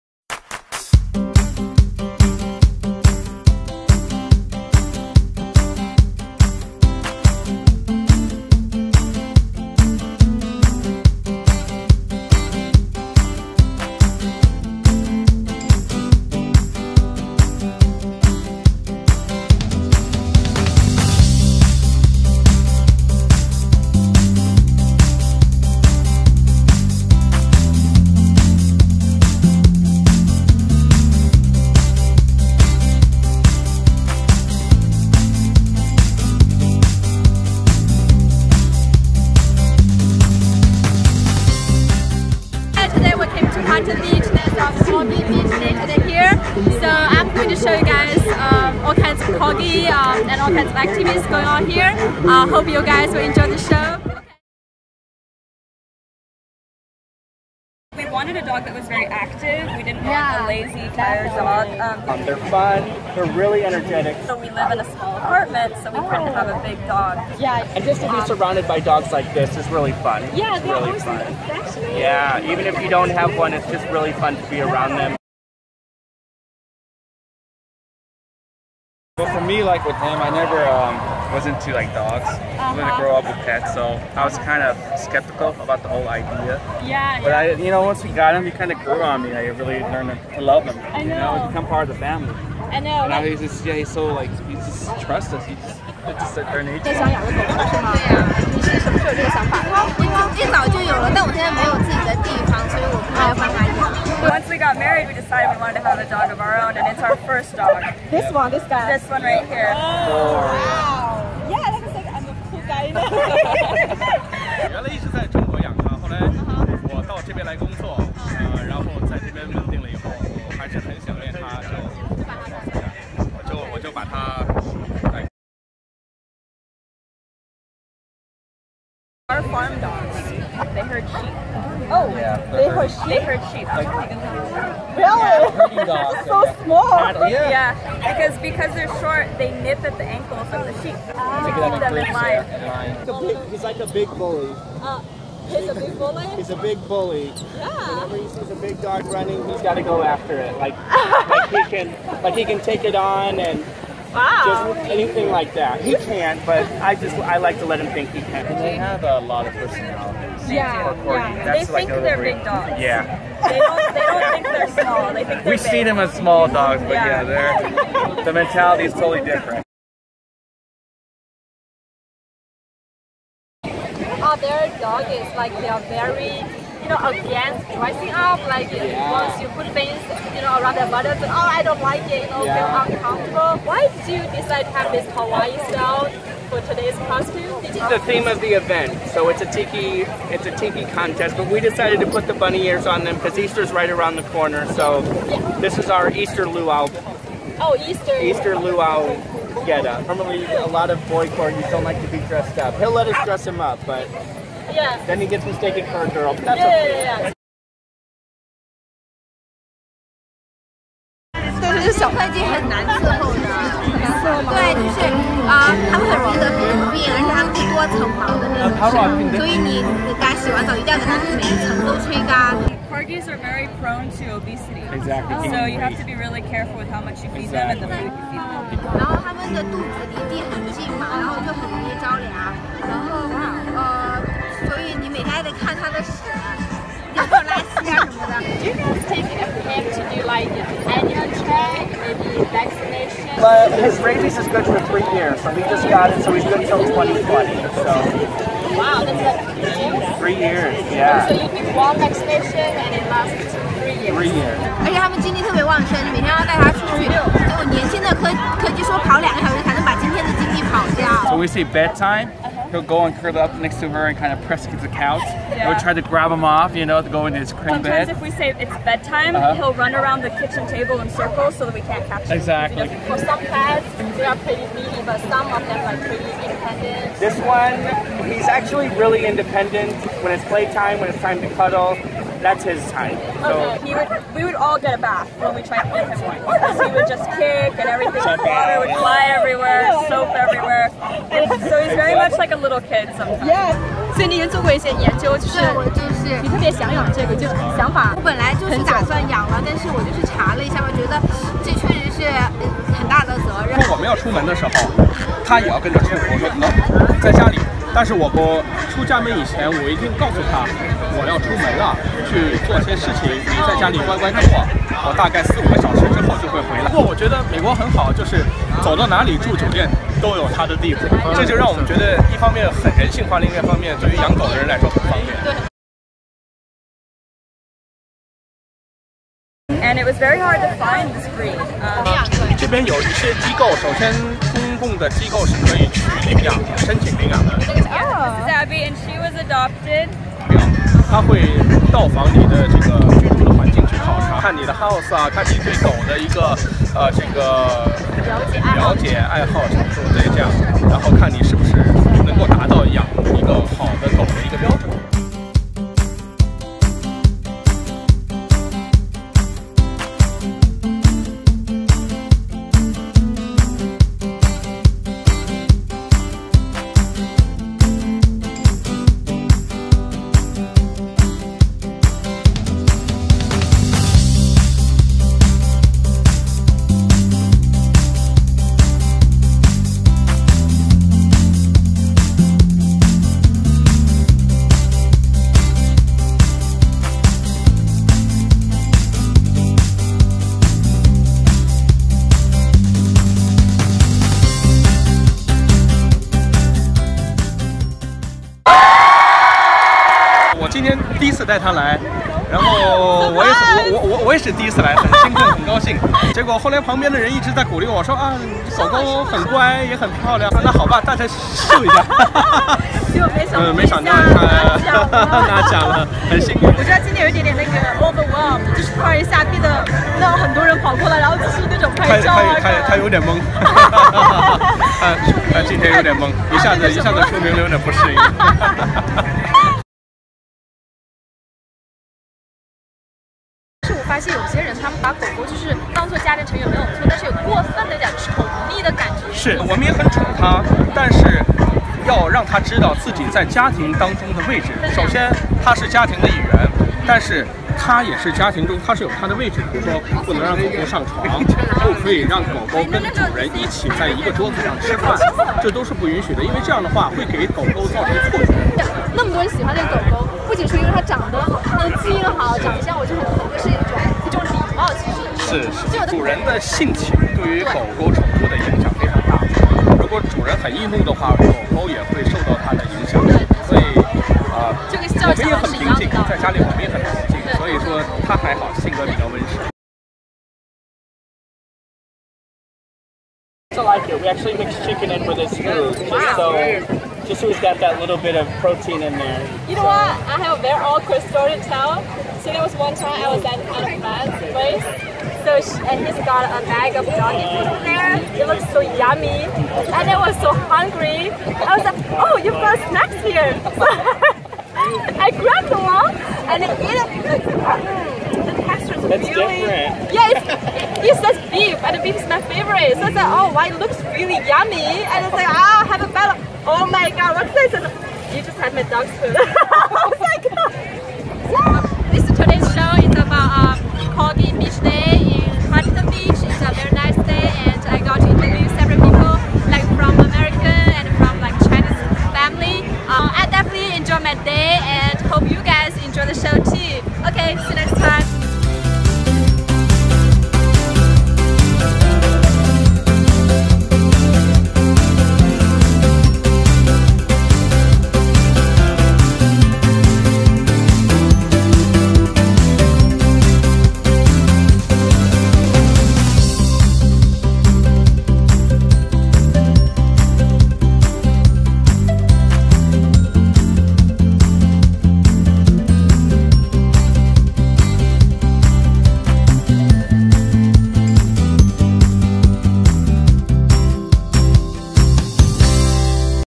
EP02: Corgi Beach Day 2017
Interviewing with corgi owners along with corgi montage!